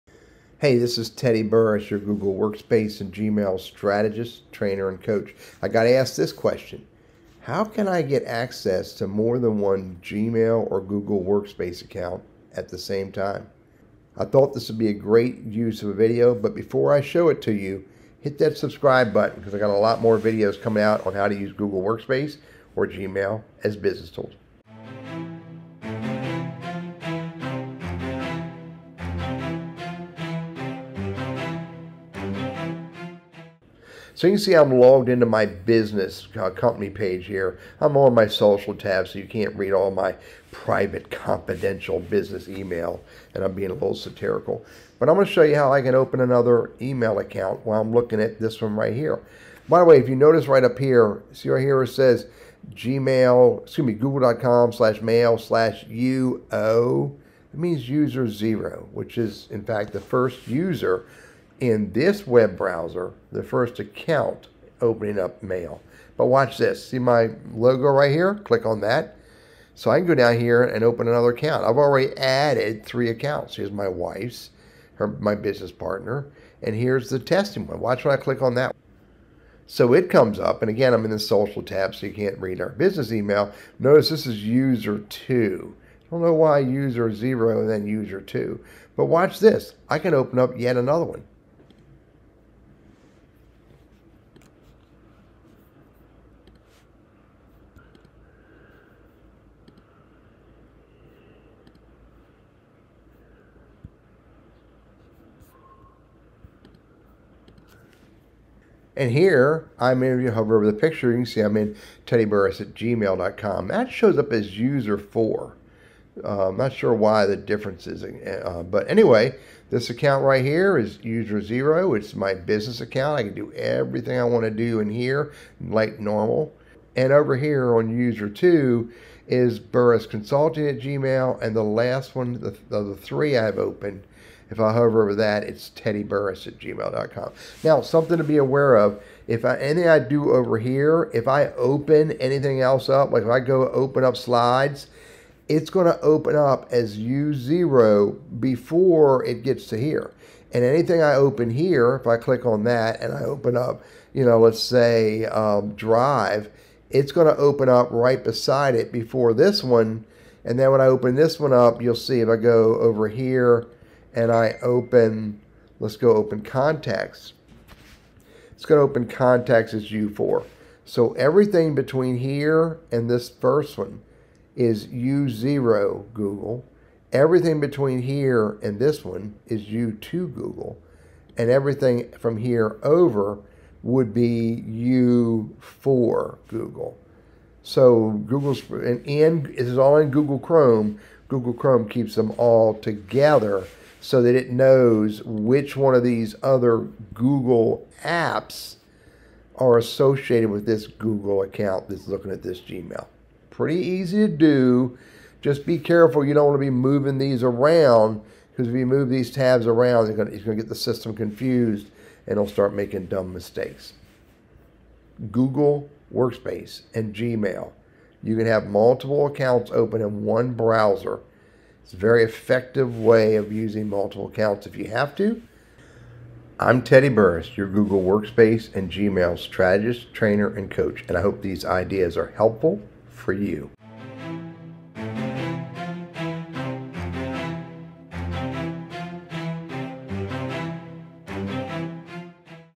How can I open multiple Google or Gmail Accounts in one Browser window
If you are using Google Chrome this is easy to do.
Just start by clicking on your Account name in the upper right corner of the browser window and select or add another account.